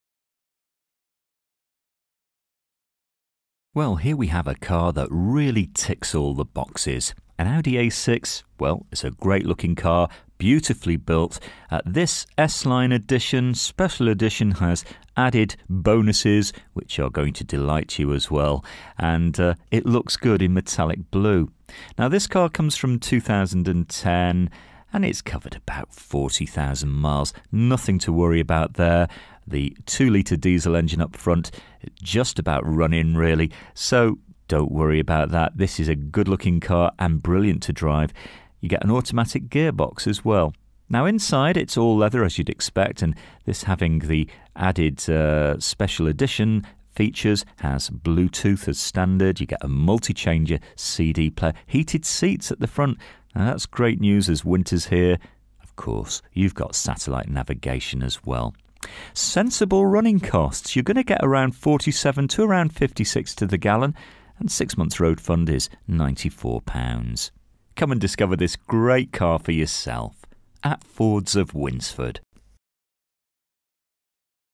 AUDI A6 AVANT SPECIAL EDITIONS (2010) 2.0 TDI 170 S LINE SPECIAL ED 5DR MULTITRONIC - LR60EAX
DETAILS:
First registered: 21/09/2010
Current mileage: 40230
Registration: LR60EAX
Colour: BLUE

FEATURES:
all round electric windows, remote central locking, all leather interior, all round airbags, climate control, pas, abs, alloy wheels, metallic paint, cd-6 multichanger, heated seats, traction control, parking control, satellite navigation, load space cover present, hand book present, cruise control, bluetooth

This used car is for sale at Fords of Winsford Car Supermarket in Cheshire, UK